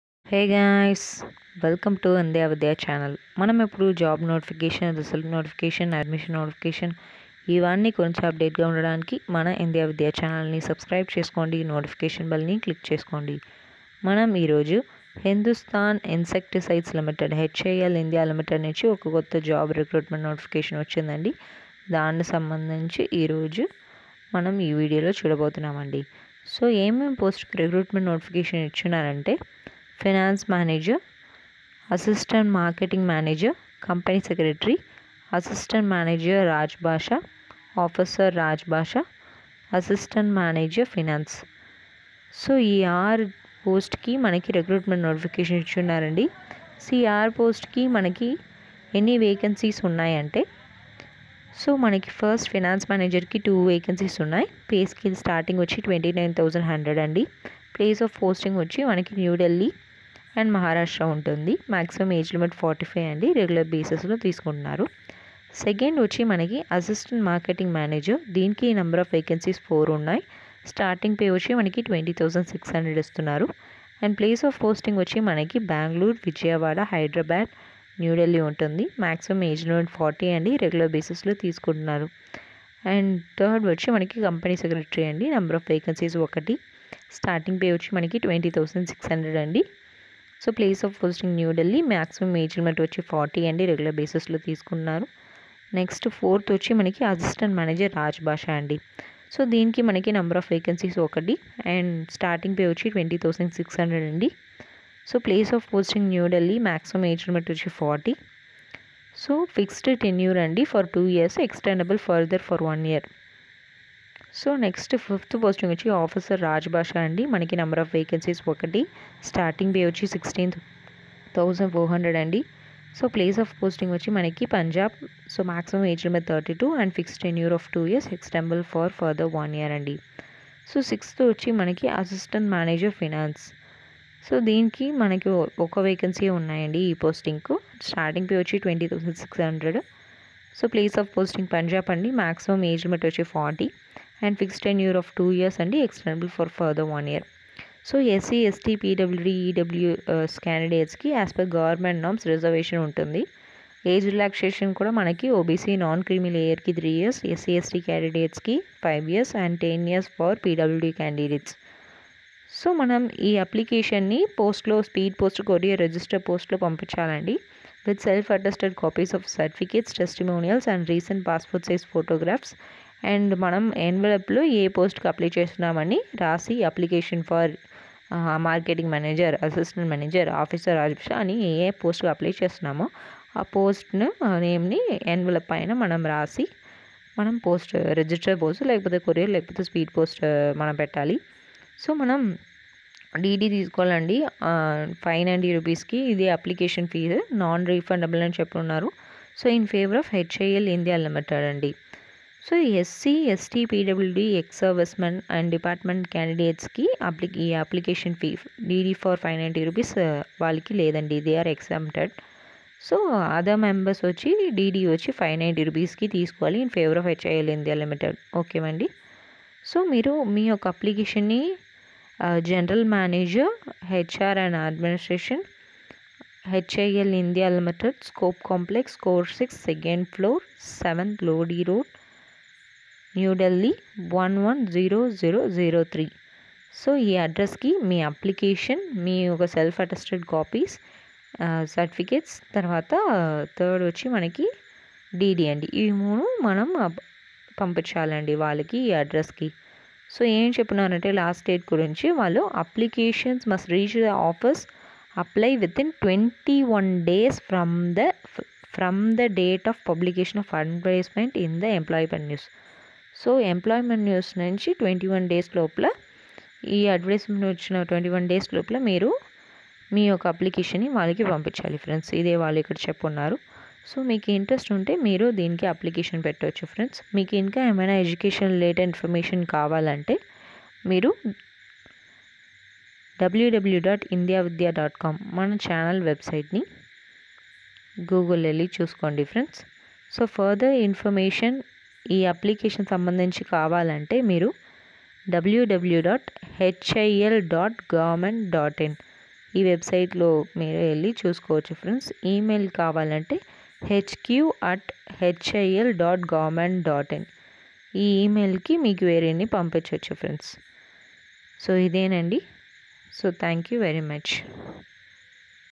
Managers and Assistants Jobs in HIL - Hindustan Insecticides Limited 2021 - Job Updates in Telugu
Hindustan Insecticides Limited Recruitment announcement 2021 for managers and assistants posts. See video for more details.